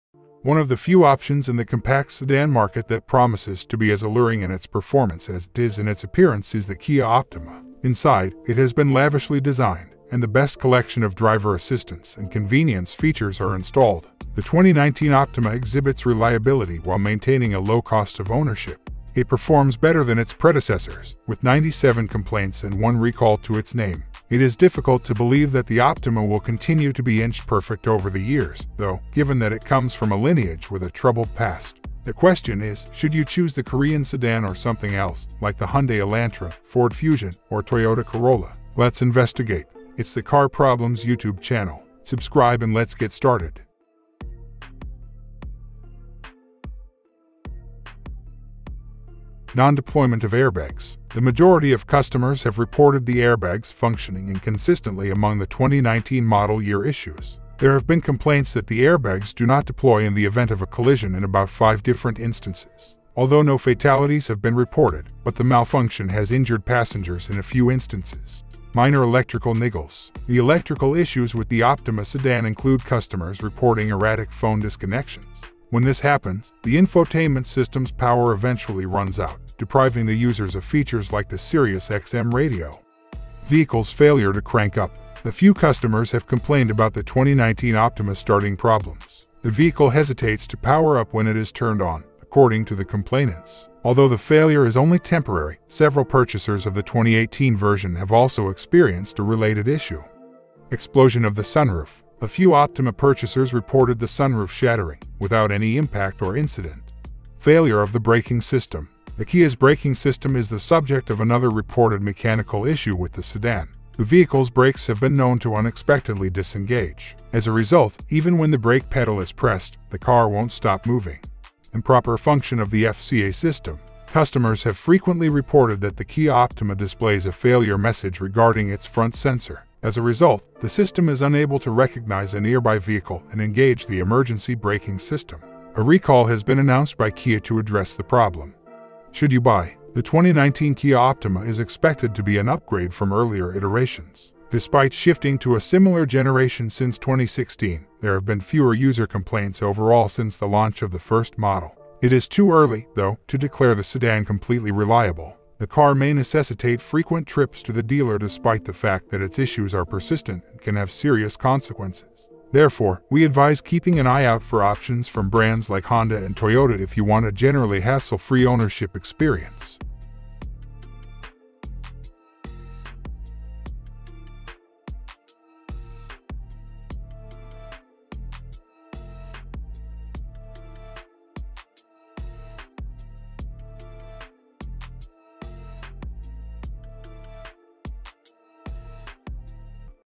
2019 Kia Optima Problems and Reliability. Should You Buy It?
The 2019 Optima exhibits reliability while maintaining a low cost of ownership.

Timecode:
00:00:00 - Blah-blah...
00:00:49 - Non-deployment of airbags
00:01:08 - Minor electrical niggles
00:01:23 - Failure to crank up
00:01:40 - Explosion of the sunroof
00:01:47 - Failure of the braking system
00:02:01 - Improper function of the FCA system
00:02:18 - Should you buy?